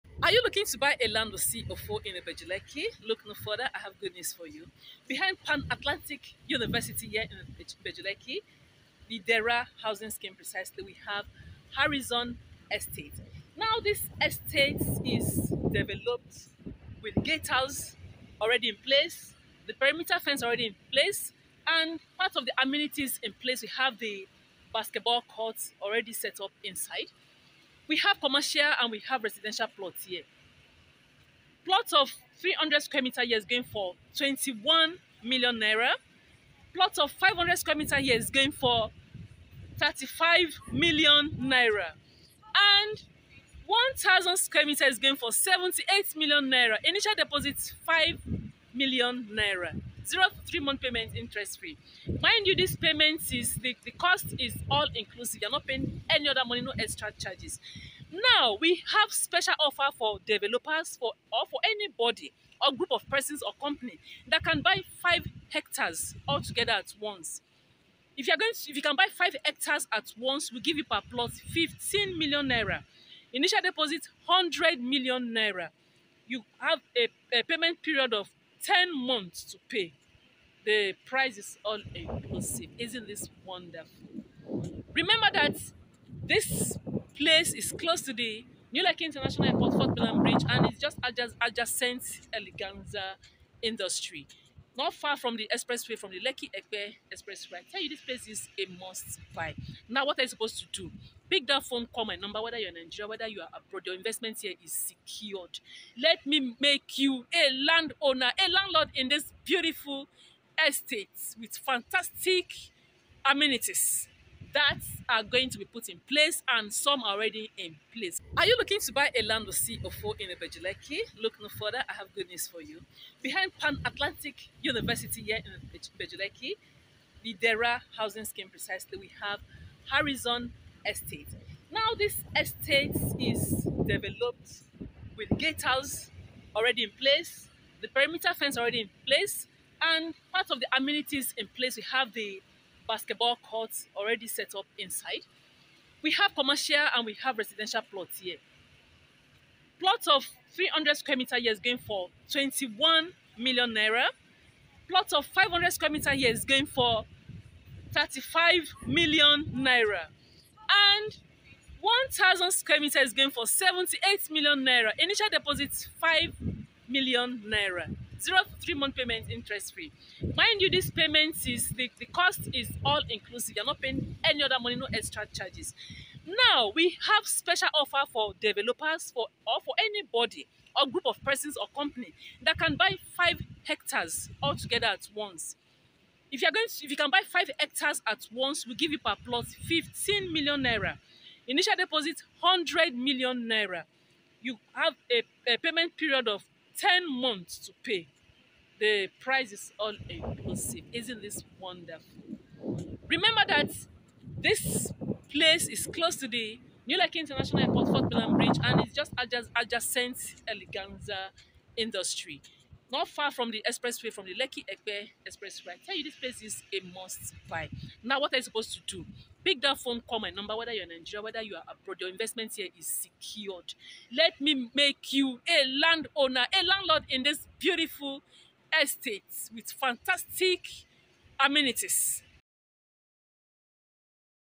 ARIZON ESTATE | Land For Sale in Ibeju Lekki Lagos
ARIZON ESTATE located in Eleko behind PAN Atlantic University Eleko Ibeju Lekki Lagos Nigeria.

Land Title : Government Allocation C of O

300SQM N21,000,000
500SQM N35,000,000

Initial Deposit N2,000,000

0 - 18 Months Payment plan available

1000SQM N78,000,000

Initial Deposit N5,000,000

Arizon Special for Estate Developers

Buy 5 hectares
N15,000,000 per plot

Initial Deposit N100,000,000

10 Months Payment plan  interest free

Get plots facing the Pan Atlantic University Road

For Inspection and purchase